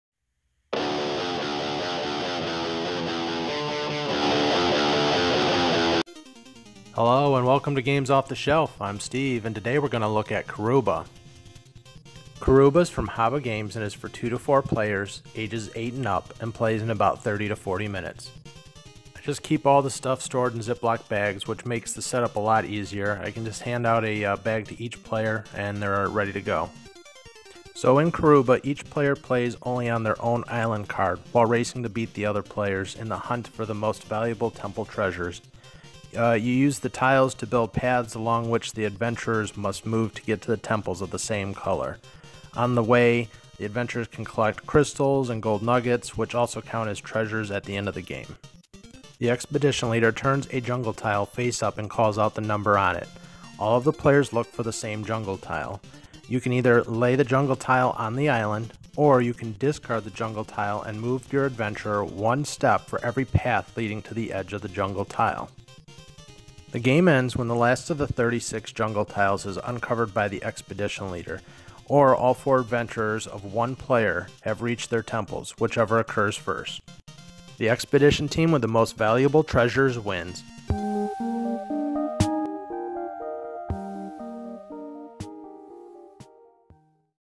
Karuba - Overview
Games Off The Shelf gives you a brief overview of Karuba from Haba games.Check out more board game overviews